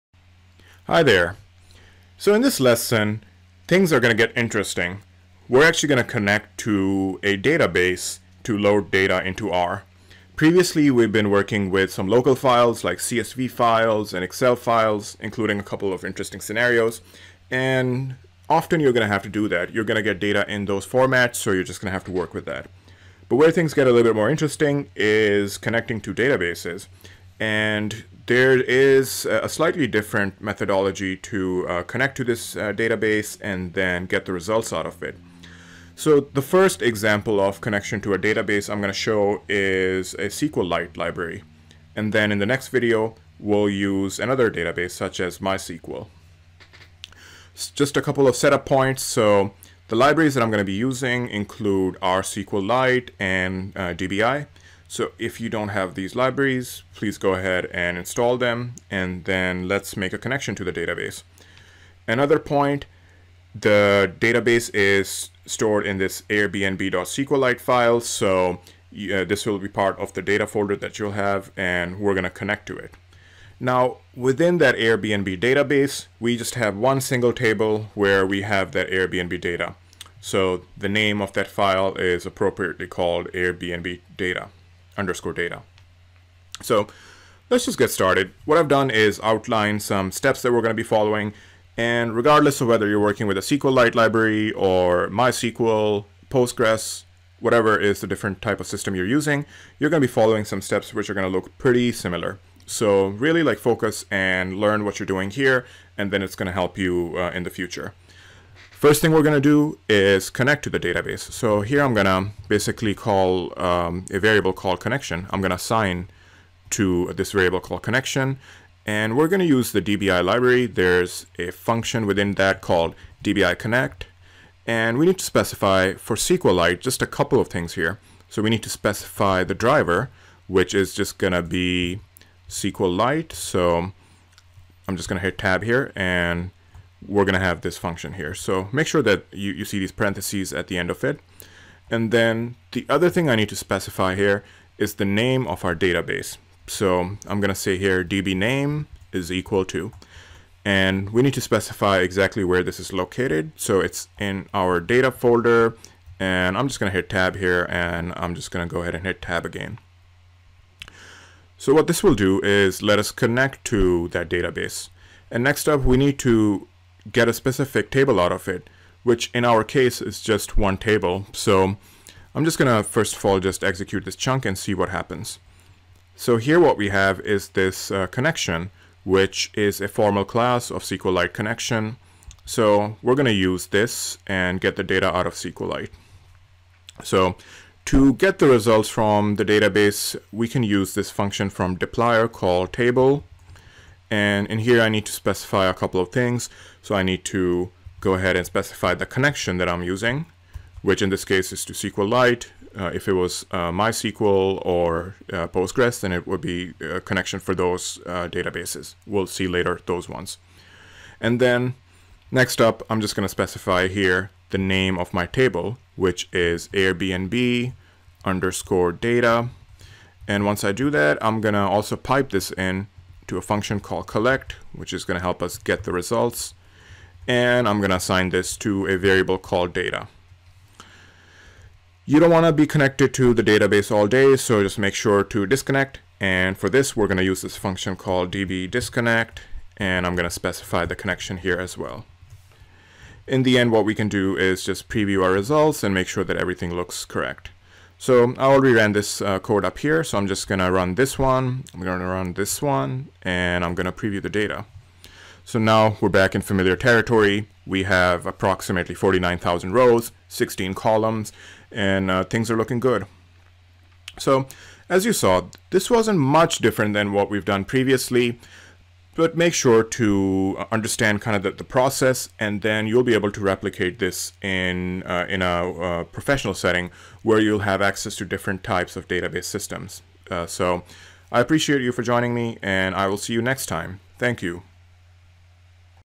Loading Data from a SQLite Database into R
Here is an example of how to load data from a SQLite database into R. We will use the RSQLite and DBI libraries to make the connection and then collect the data into memory.